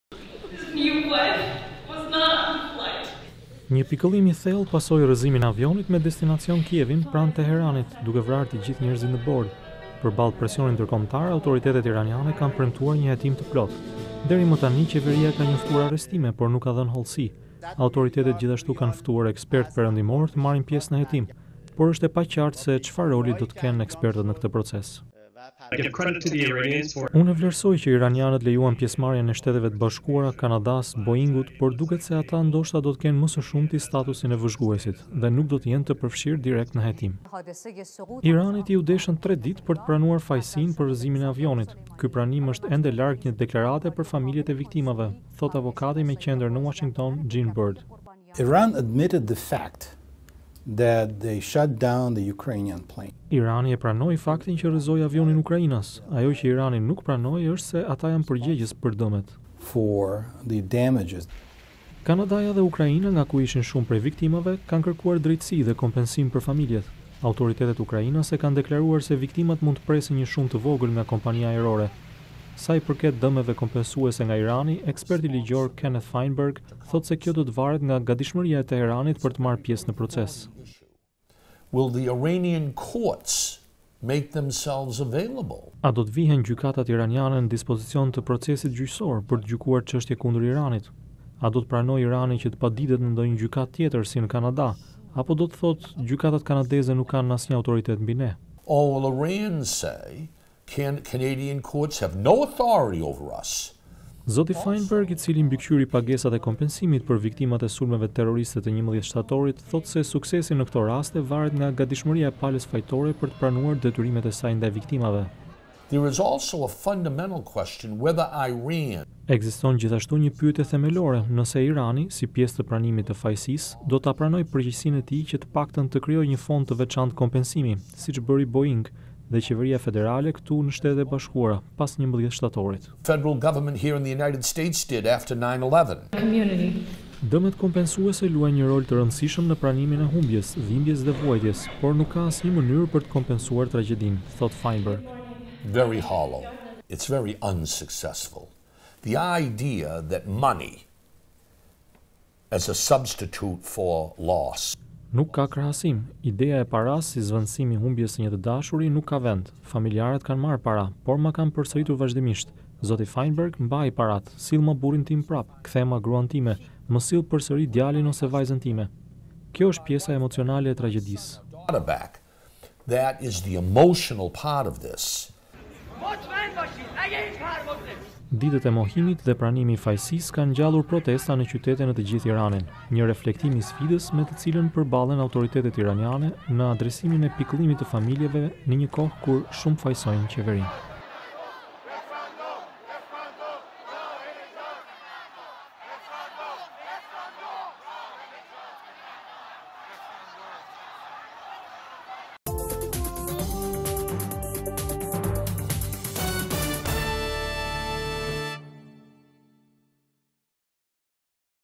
Kompensimi i viktimave të avionit ukrainas
Pranimi i përgjegjësisë nga Irani për rrëzimin e avionit ukrainas të pasagjerëve që la 176 të vdekur, ka ngjallur zemërim dhe mosbesim tek familjet e viktimave dhe ka shtuar thirrjet ndërkombëtare për përgjegjësi. Ndërsa familjet vajtojnë të dashurit, ka diskutime për kompensim monetar për humbjen e tyre.